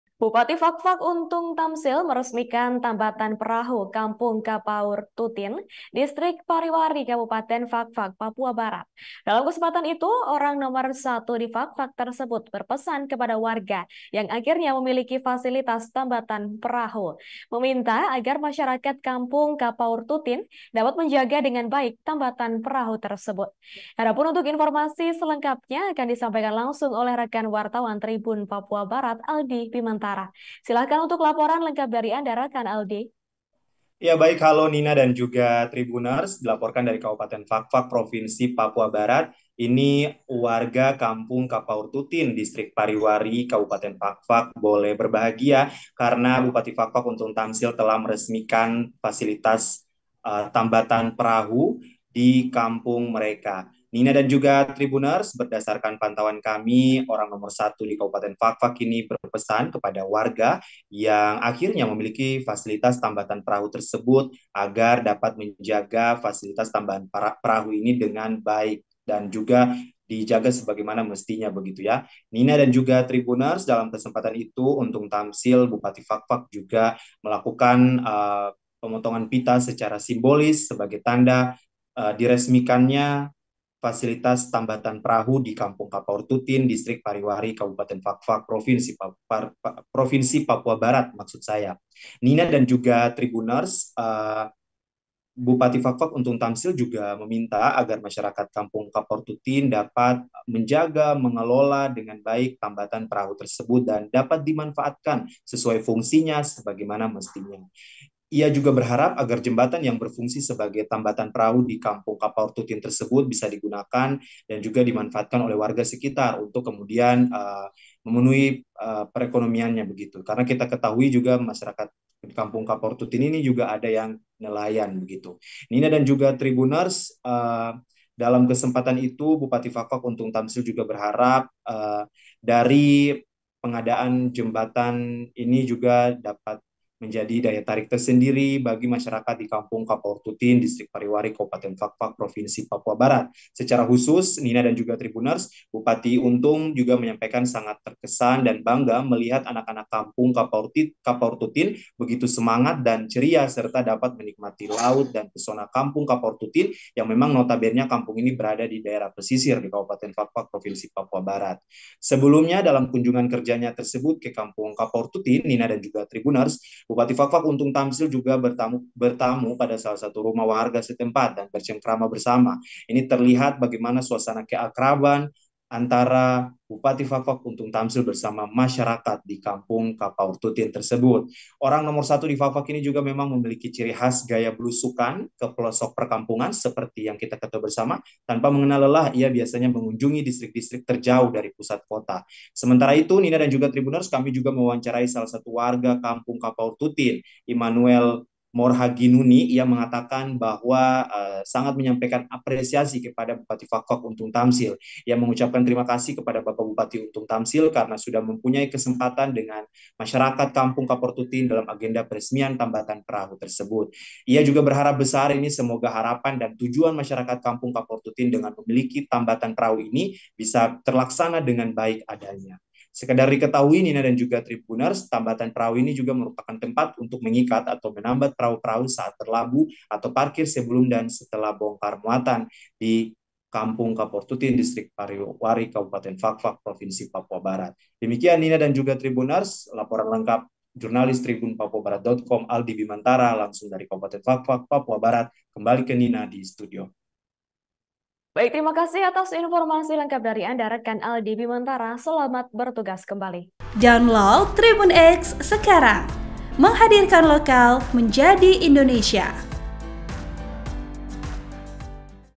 Bupati Fakfak Resmikan Tambatan Perahu Kampung Kapaurtutin, Imbau untuk Menjaga & Gunakan Semestinya
Laporan Wartawan Tribun Papua Barat, Aldi Bimantara

TRIBUN-VIDEO.COM - Bupati Fakfak Untung Tamsil meresmikan tambatan perahu Kampung Kapaurtutin, Distrik Pariwari Kabupaten Fakfak Papua Barat.

Dalam kesempatan itu, orang nomor satu di Fakfak itu berpesan kepada warga yang akhirnya memiliki fasilitas tambatan perahu tersebut.

"Alhamdulillah, Puji Tuhan bersama masyarakat Kampung Kapaurtutin, saya meresmikan tambatan perahu yang telah lama dinanti," ujar Untung kepada TribunPapuaBarat.com di Fakfak, Minggu (3/12/2023).

Ia meminta agar masyarakat Kampung Kapaurtutin dapat menjaga dengan baik tambatan perahu itu.

Untung berharap agar jembatan yang berfungsi sebagai tambatan perahu di Kampung Kapaurtutin tersebut, bisa digunakan sebagaimana mestinya.

(Tribun-Video.com/TribunPapuaBarat.com)